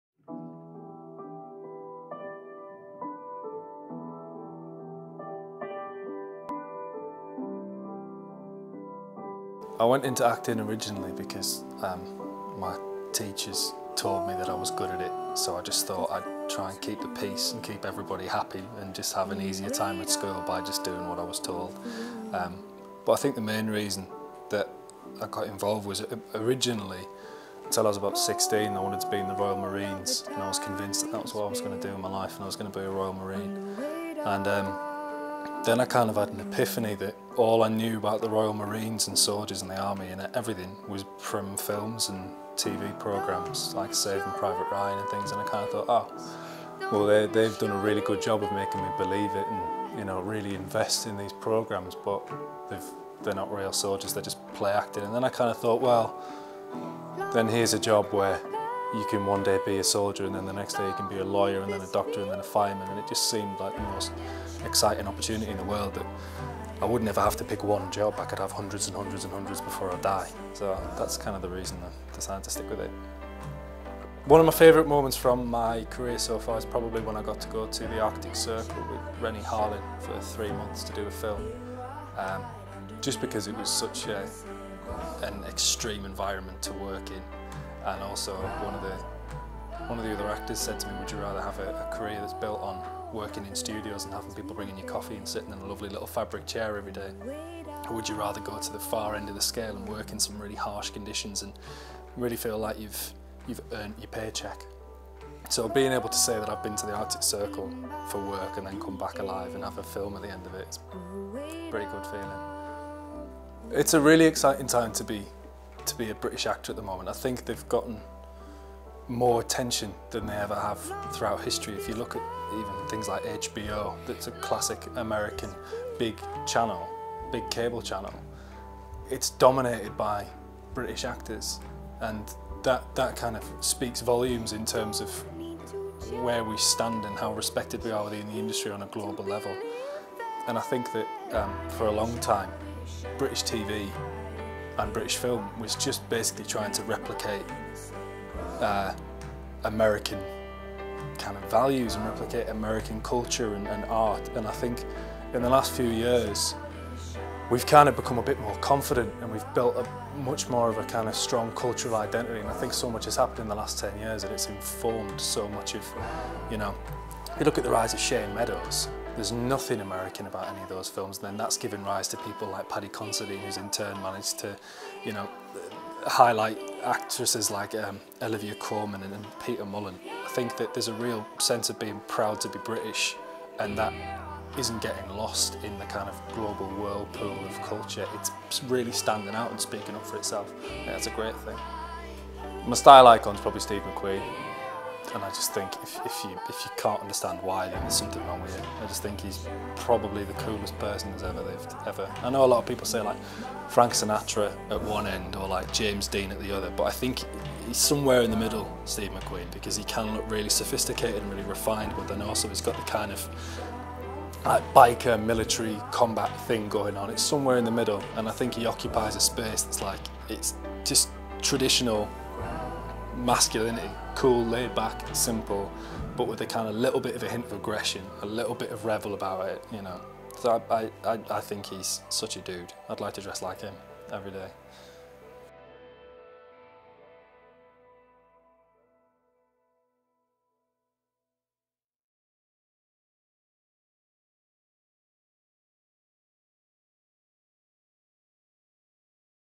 Matt Stokoe Interview - The Young Thespians
The young up-and-coming British actors of 2013, talk projects, style, and experiences in this '1920s meets 2013' themed shoot.